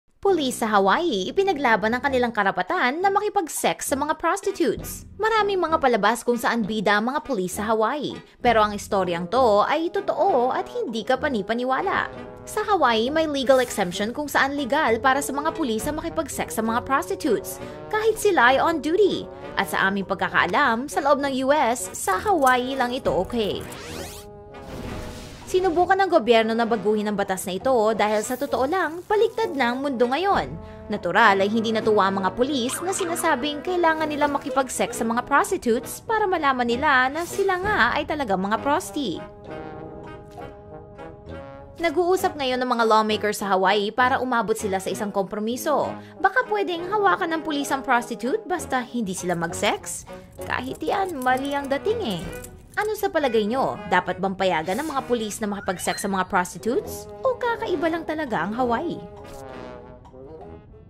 Pulis sa Hawaii, ipinaglalaban ang kanilang karapatan na makipag-sex sa mga prostitutes!
Pulis sa Hawaii, ipinaglaban ang kanilang karapatan na makipag-sex sa mga prostitutes!

Maraming mga palabas kung saan bida ang mga pulis sa Hawaii. Pero ang istoryang ito ay tutoo, at hindi kapani-paniwala.

Sa Hawaii, may legal exemption kung saan legal para sa mga pulis ang makipag-sex sa mga prostitutes. Kahit sila ay on duty! At sa aming pagkakaalam, sa loob ng US, sa Hawaii lang ito okay.

Sinubukan ng gobyerno na baguhin ang batas na ito, dahil sa tutoo lang, baligtad na ang mundo ngayon! Natural, ay hindi natuwa ang mga pulis, na sinasabing kailangan nilang makiapg-sex sa mga prostitutes, para malaman nila na sila nga ay talagang mga prosti.

Nag-uusap ngayon ang mga lawmakers sa Hawaii, para umabot sila sa isang kompromiso...baka pwedeng hawakan ng pulis ang prostitute, basta hindi sila mag-sex? Kahit iyan, mali ang dating eh.

Ano sa palagay niyo -- dapat bang payagan ang mga pulis na makipag-sex sa mga prostitutes, o kakaiba lang talaga ang Hawaii?